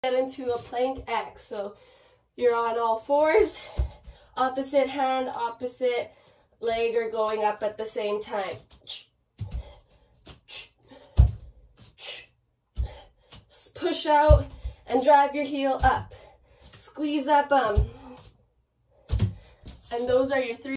X Plank
1. Start by placing hands a bit wider then shoulder width and feet out wide, on the balls of your feet.
2. Engage your core by tucking your belly button to your tail bone
3. Raise opposite arm and opposite leg in the air, squeezing your bum and heal back, as you reach your arm and control your body as you bring back to mat.
4.Switch to the other side. Repeate.